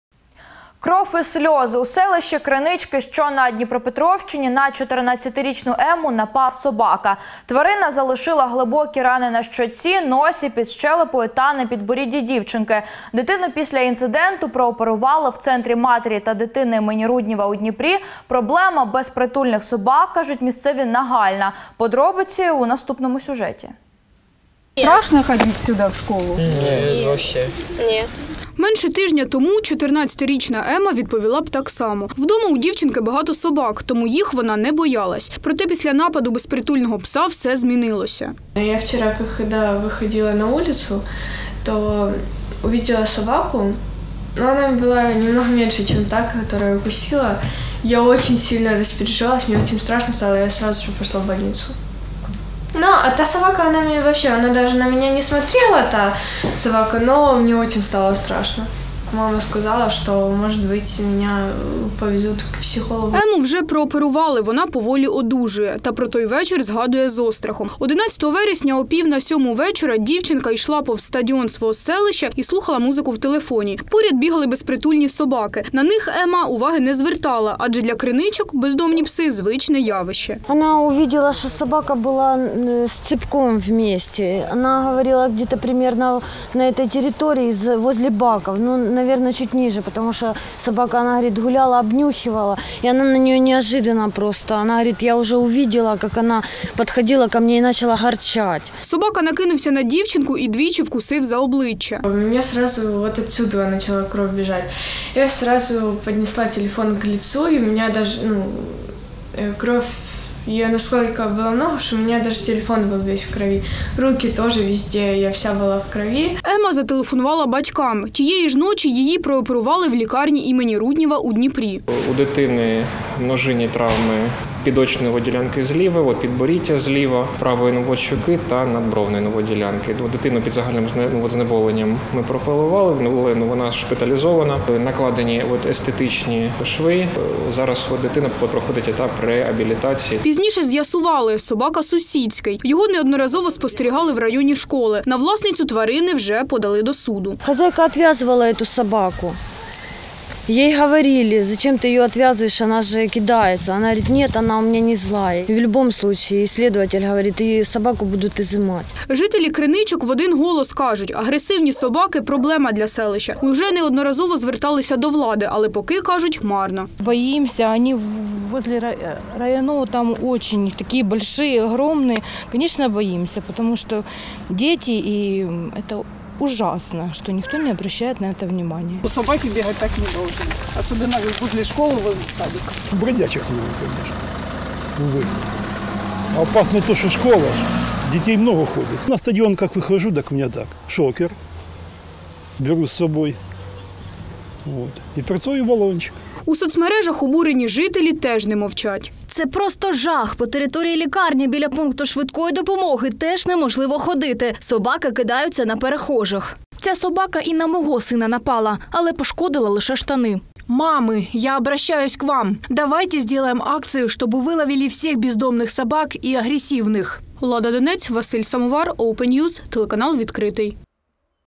На Дніпропетровщині безпритульний собака вкусив дівчинку за обличчя
Кров і сльози. У селищі Кринички, що на Дніпропетровщині, на 14-річну Емму напав собака. Тварина залишила глибокі рани на щоці, носі, під щелепою та на підборідді дівчинки. Дитину після інциденту прооперували у Центрі матері та дитини ім. Руднєва Дніпра. Проблема безпритульних собак, кажуть місцеві, – нагальна _______________________________________________